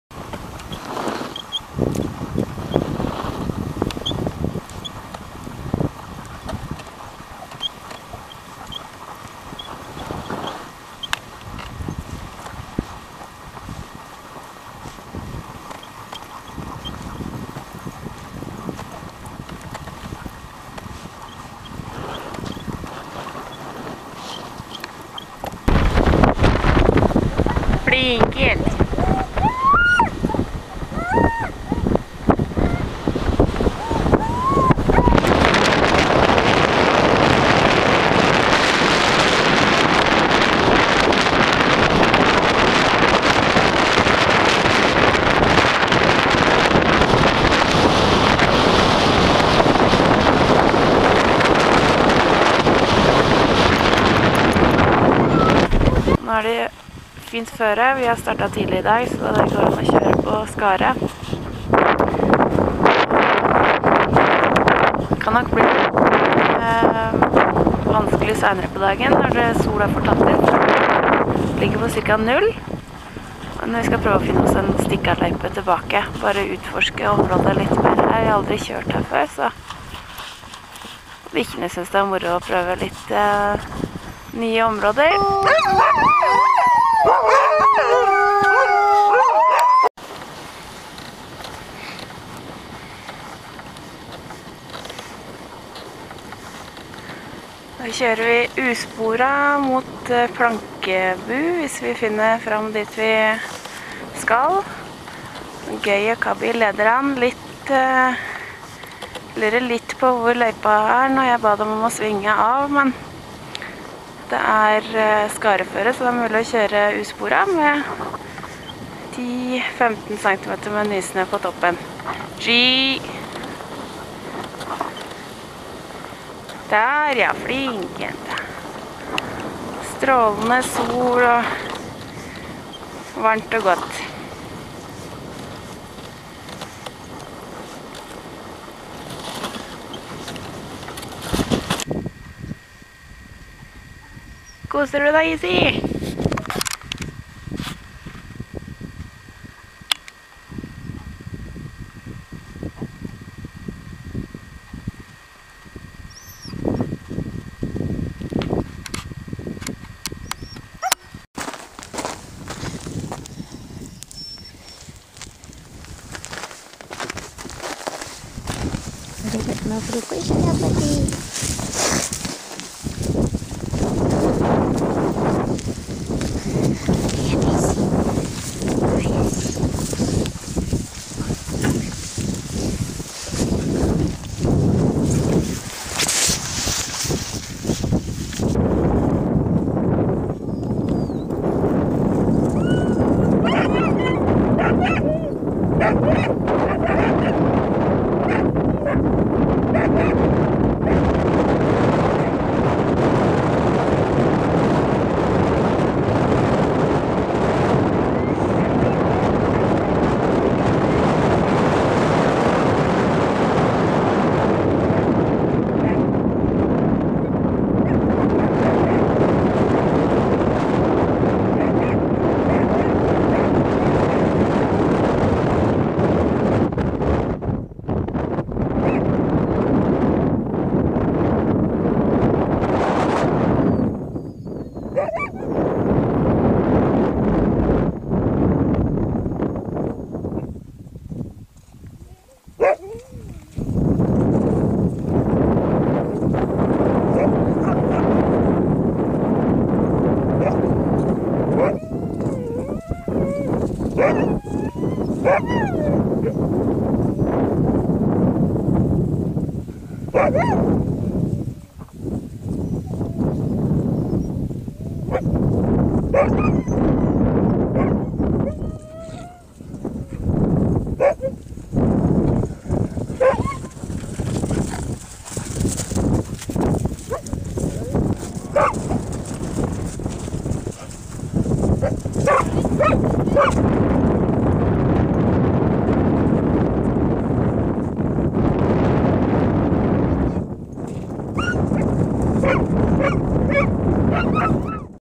Vårturer 2012
Av og til har vi med oss videokamera på tur. Her er noen småklipp fra ulike turer i vårfjellet.